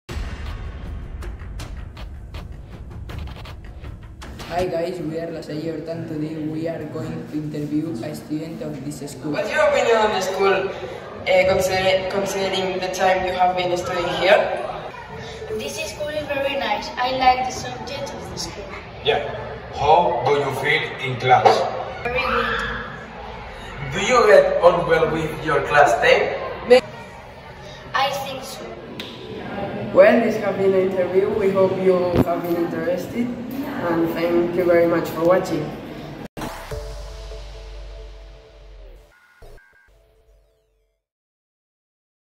INTERVIEW 4th ESO with 6th EP
Watch this interview that the 4thof ESO students have conducted with a 6th grade pupil in order to get to know a little bit about their primary schoolmates. This is the result of the work they have carried out along several sessions in Hora Complementària Anglès. Good job, guys!